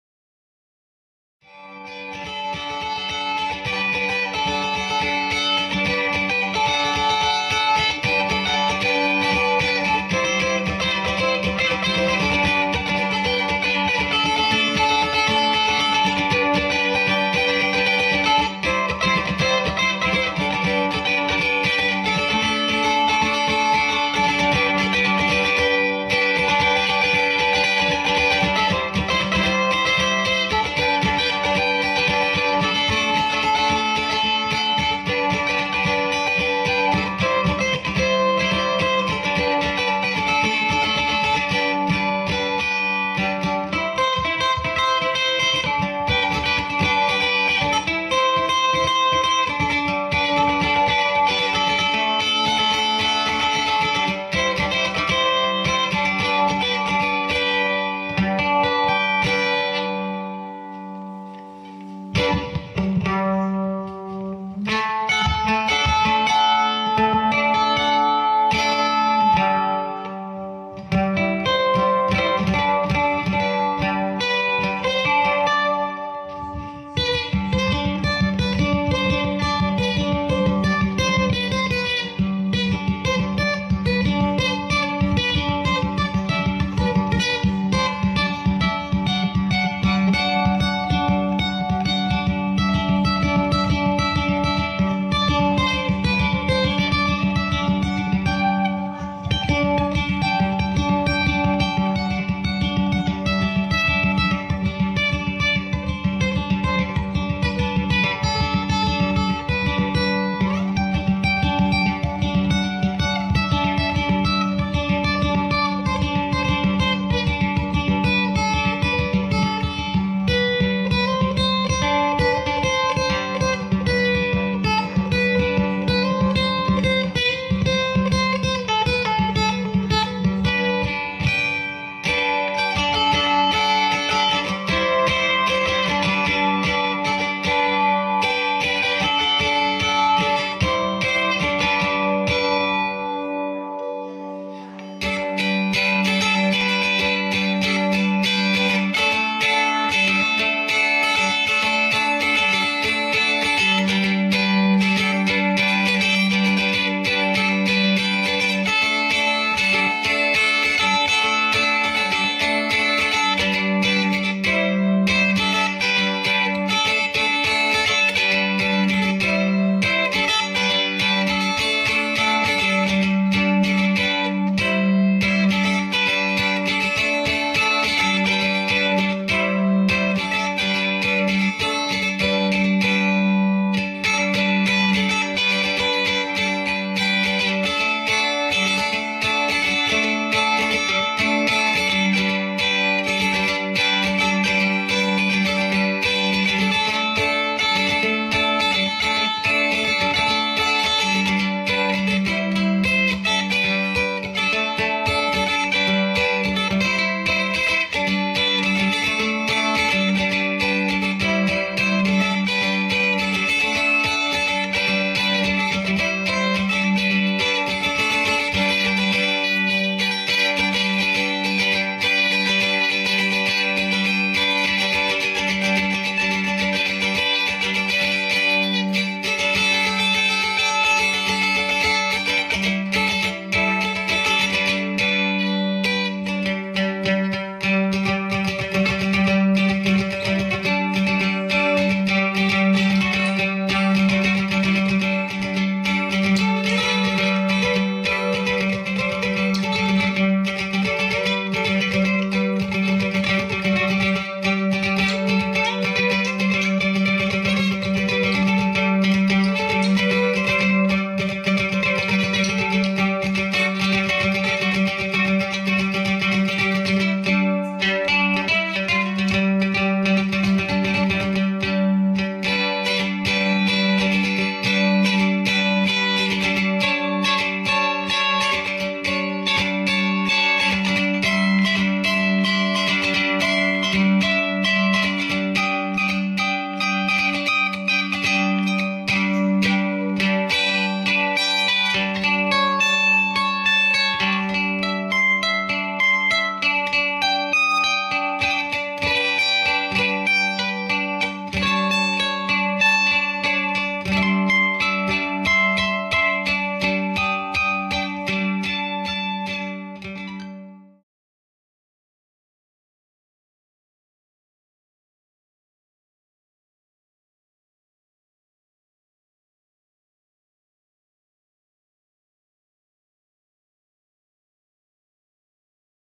JON PLAYS A MANDOLIN THROUGH THE NACE M1-18R
JON PLAYS A MANDOLIN THROUGH THE NACE M1-18R WITH THE EMINENCE GB128 SPEAKER.